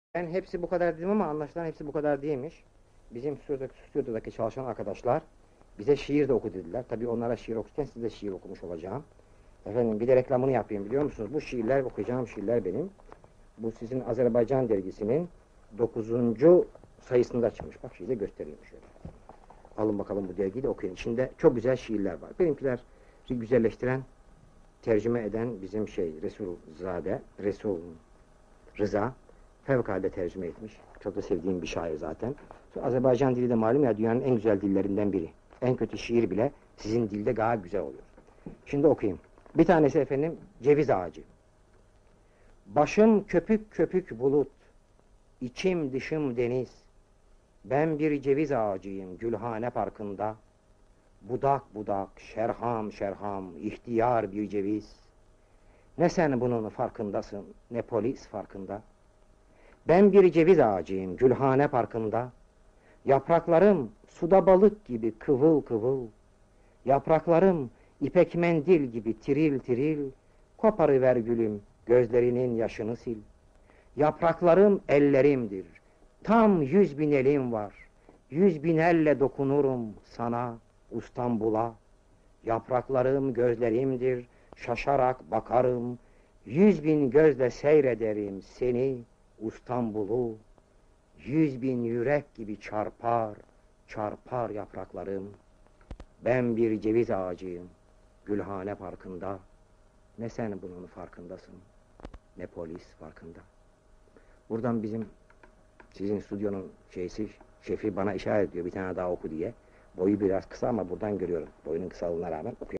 Nazım Hikmet, "Ceviz Ağacı" Şiirini Okuyor
CEVİZ AĞACI
Başım köpük köpük bulut, içim dışım deniz,
ben bir ceviz ağacıyım Gülhane Parkı'nda,
budak budak, şerham şerham ihtiyar bir ceviz.
Ne sen bunun farkındasın, ne polis farkında.

Yapraklarım suda balık gibi kıvıl kıvıl.
Yapraklarım ipek mendil gibi tiril tiril,
koparıver, gözlerinin, gülüm, yaşını sil.
Yapraklarım ellerimdir, tam yüz bin elim var.
Yüz bin elle dokunurum sana, İstanbul'a.
Yapraklarım gözlerimdir, şaşarak bakarım.
Yüz bin gözle seyrederim seni, İstanbul'u.
Yüz bin yürek gibi çarpar, çarpar yapraklarım.